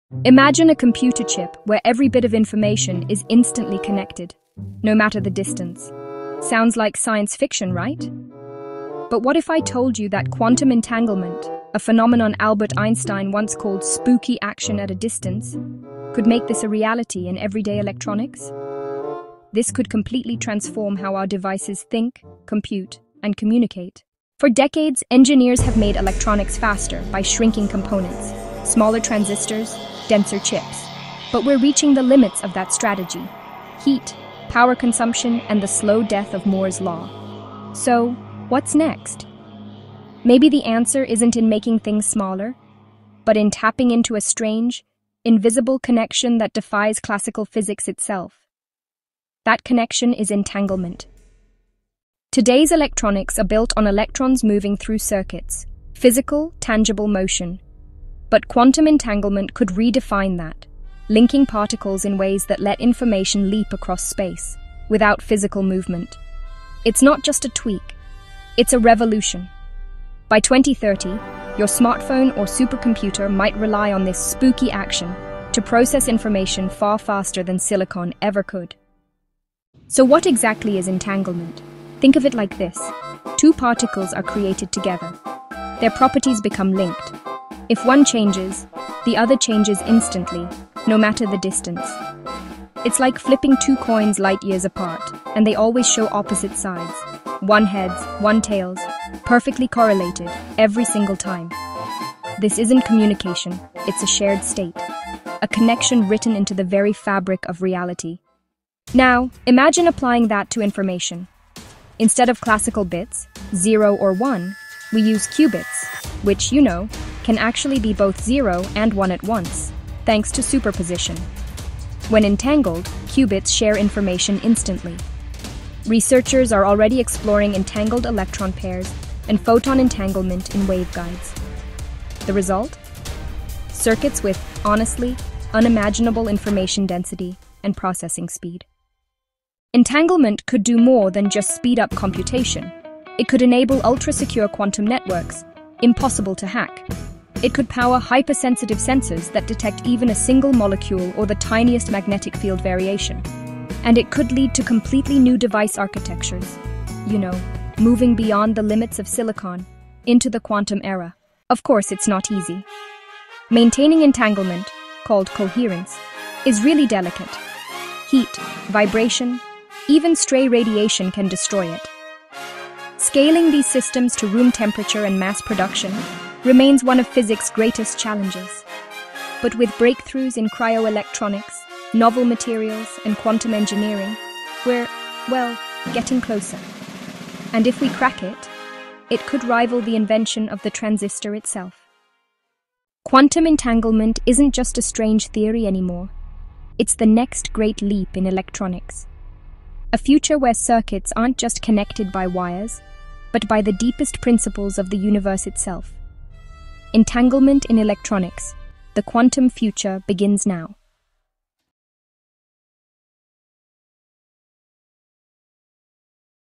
Quantum Entanglement: The Future of Electronics? | Revolution in 2025 Explained
2025 marks the International Year of Quantum Science and Technology (IYQ), and the future of electronics is already being rewritten! This video dives into the mind-bending reality of Quantum Entanglement—Einstein's "spooky action at a distance"—and its revolutionary impact on the next generation of technology.

From Quantum Computing that will shatter today's encryption methods to ultra-secure Quantum Communication and sensors with unprecedented precision, entanglement is the crucial ingredient. We break down:

⚛️ The Science: What is entanglement and why is it the backbone of the quantum realm?

🏆 The Breakthroughs: How did the work of the 2025 Nobel laureates for macroscopic quantum effects pave the way for real-world quantum chips?

💻 The Applications: See how entangled qubits are enabling quantum advantage for industries like finance, pharmaceuticals, and AI.

🔒 The Threat & The Promise: The race for post-quantum cryptography versus the potential for an unhackable quantum internet.

Join us to explore how quantum entanglement is transitioning from a physics oddity to the foundation of tomorrow's electronic world.